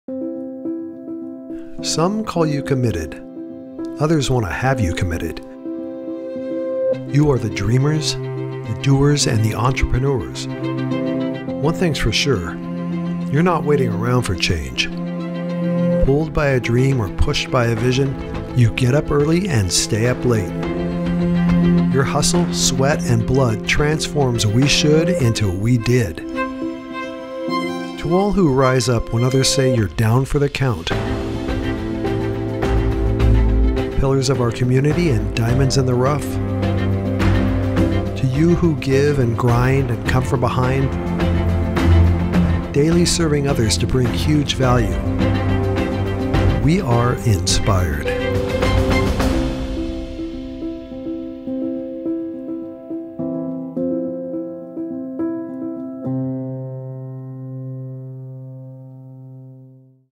You Inspire Us!
Check out our tribute to the dreamers, the doers and the entrepreneurs. This video is our way of honoring the amazing people here in our home town of Vancouver, Washington that make it such a special place to live, work, and play!